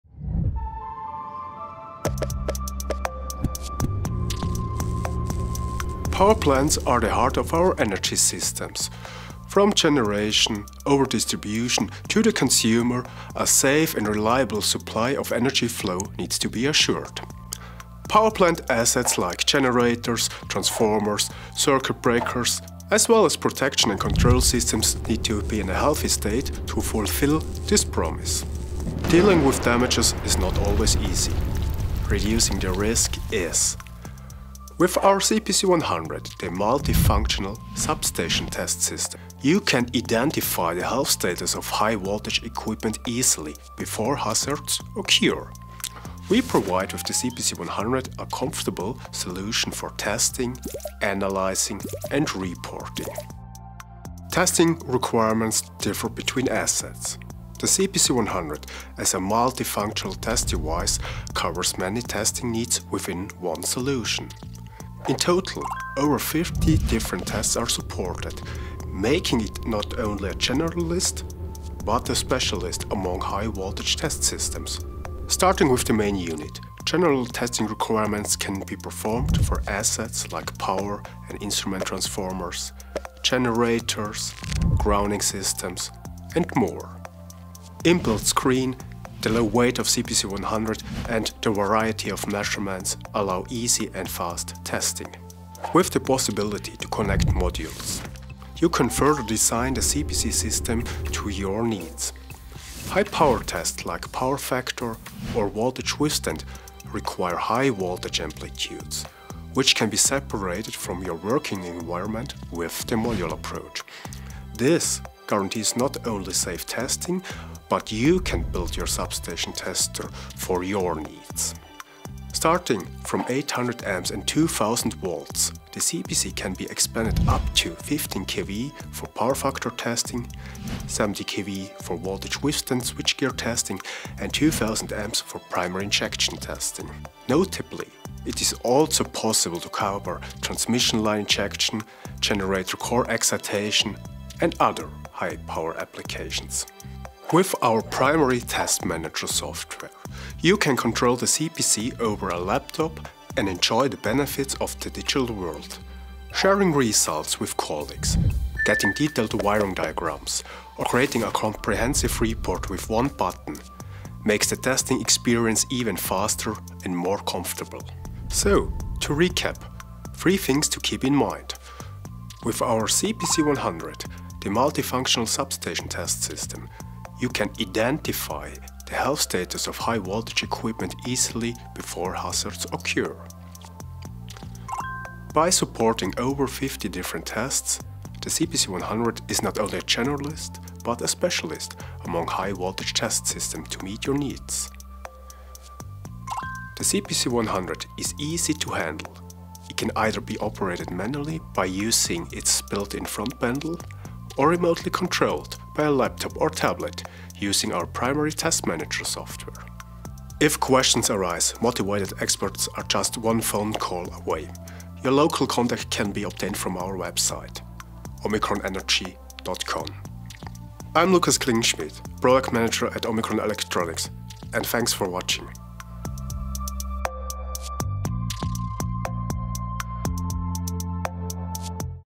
CPC 100 - Why is it important to do asset maintenance ?
Lukas Klingenschmid, product manager for our CPC 100 multifunctional test system, gives an insight about the importance of asset maintenance.

Power plant assets like generators, transformers, circuit breaker as well as protection and control systems need to be in healthy state to fulfill this promise. He explains the advantages of CPC 100, the multifunctional substation test system to identify the health status of high-voltage equipment easily, before hazards occur.

TIMESTAMPS:
00:00 – Importance of asset maintenance
00:33 – CPC 100 test system
01:04 – Testing solutions
01:36 – Modular test system
02:27 – PTM Software
02:48 – Recap
03:30 – Outro

Would you like to find out more about OMICRON, our passions, and our products?